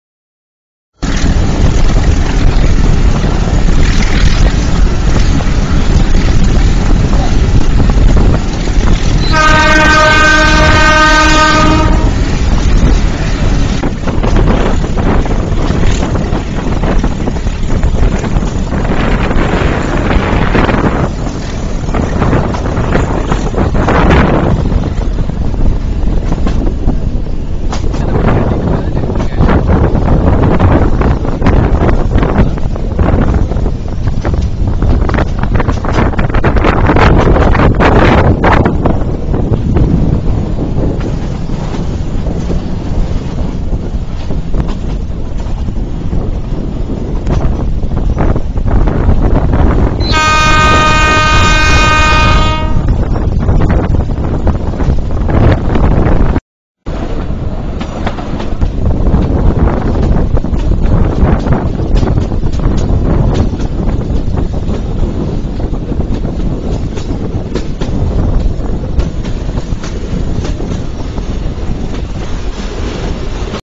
Madurai - Bodi Meter Gauge train entering Theni
This service was closed down for Gauge conversion. But still the project is going on for a long time.